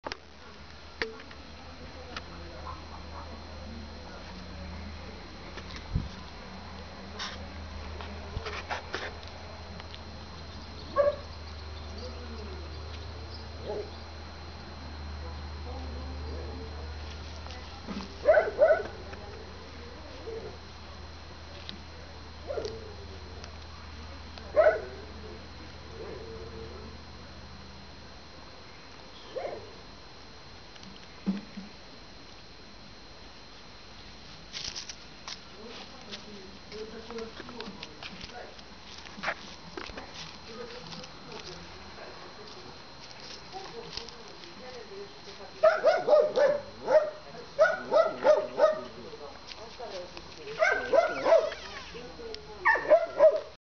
Papi cocker spaniel
Papi at 5 months old playing at countryside. He loves to explore everything, and i love him so much ;))